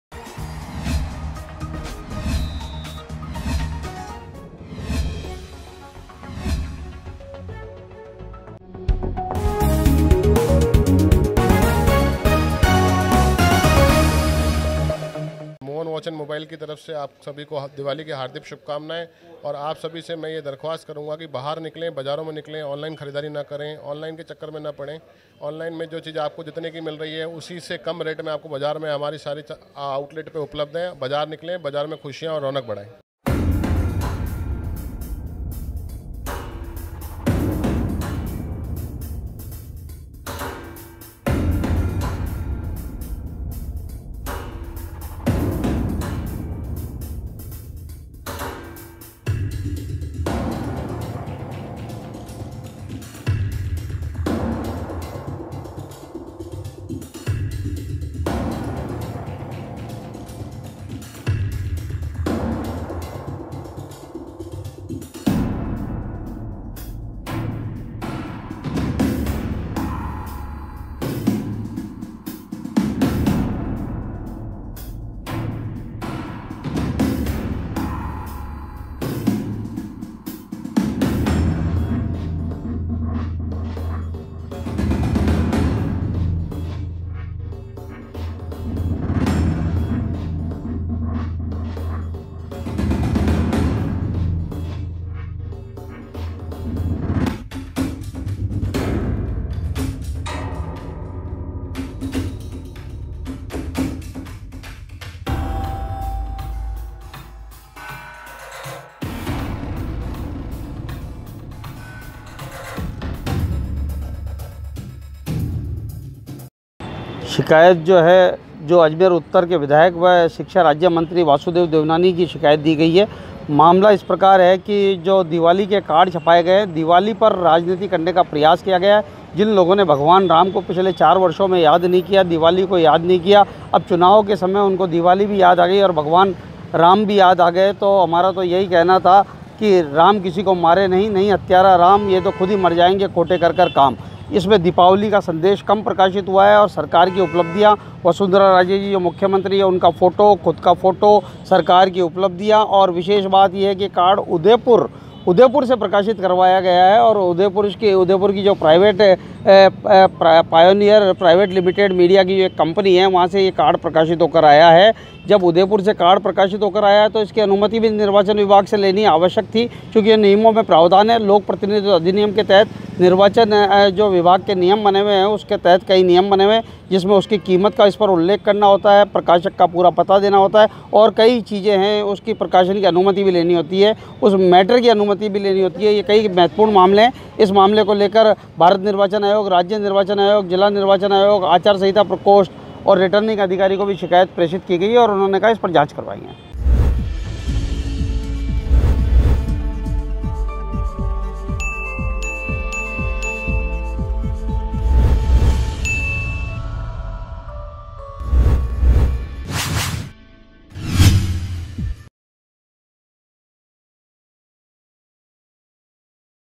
अजमेर में वासुदेव देवनानी के खिलाफ आचार संहिता का उल्लंघन का आरोप | MTTV INDIA
वासुदेव देवनानी के खिलाफ आचार संहिता का उल्लंघन का आरोप

वकील विवेक पाराशर ने निर्वाचन आयोग को शिकायत दी